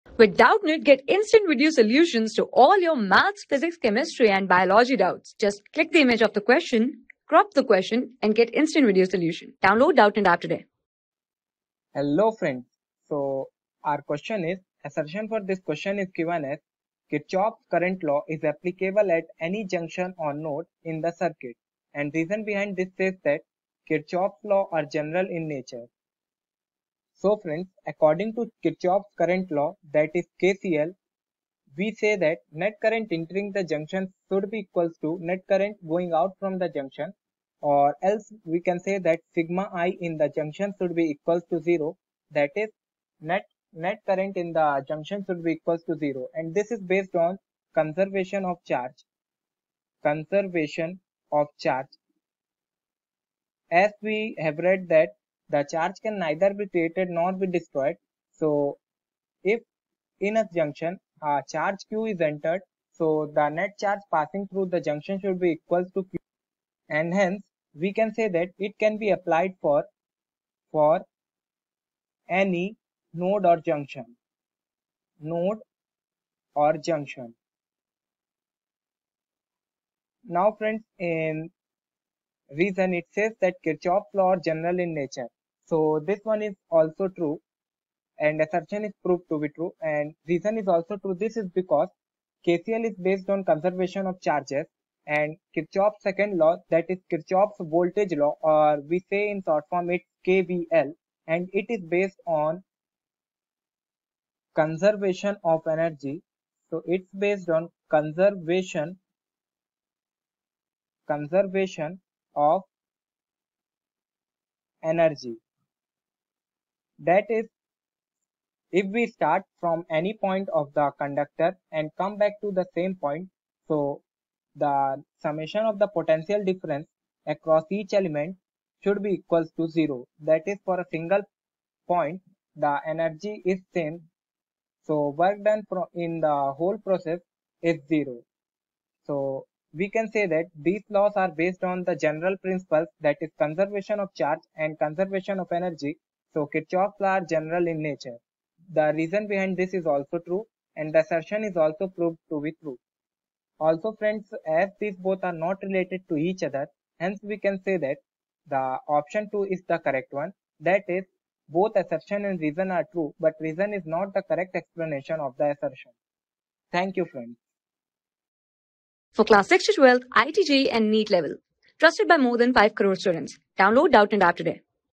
A : Kirchhoff\'sCurrentlaw is applicableat anyjunctionisor nodein the circuit. R : Kirchhoff\'s ...
A : Kirchhoff\'sCurrentlaw is applicableat anyjunctionisor nodein the circuit. R : Kirchhoff\'s laws are general in nature .
Class: 12
Subject: PHYSICS
Chapter: CURRENT ELECTRICITY
Board:IIT JEE

👉 Have Any Query? Ask Us.